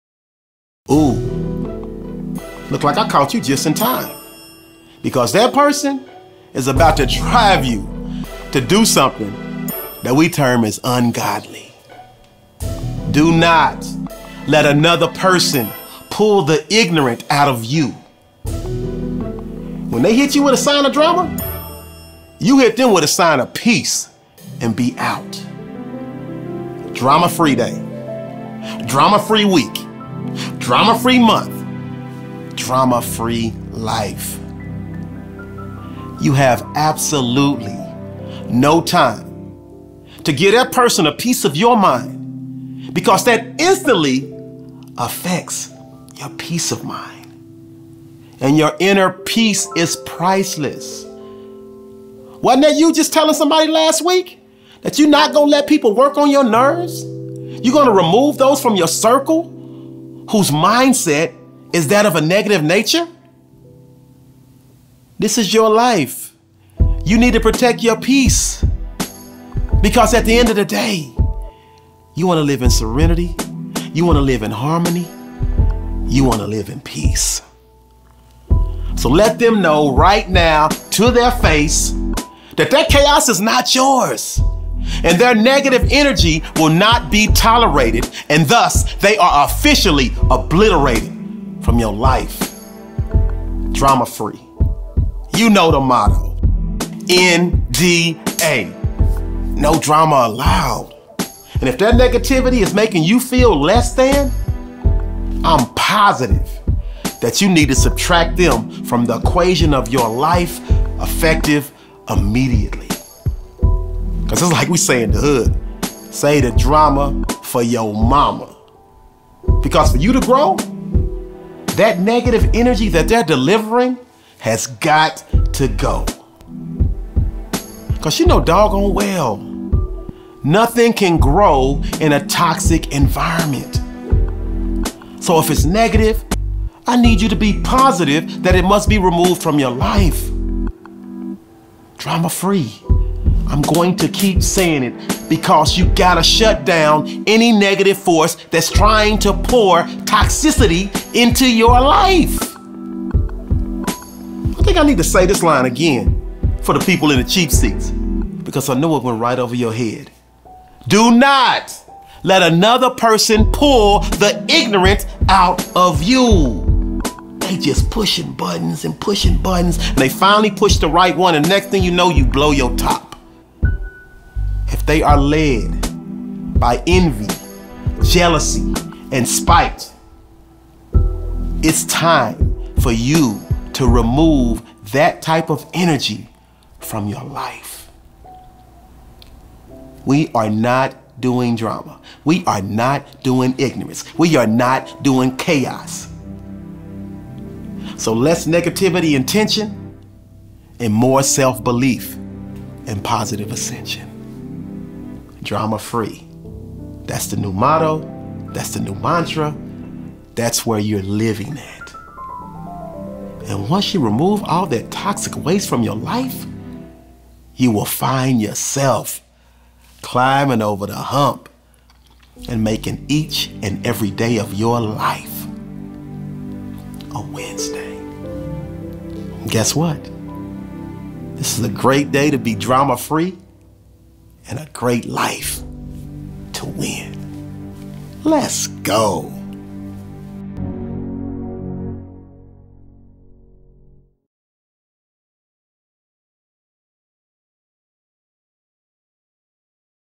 How To Live A Drama Free Life | Daily Motivation Dose | Freddy Fri
How to live a drama free life daily motivation dose from motivational speaker Freddy Fri

This episode is designed to help you eliminate toxic energy so you can live a drama free life!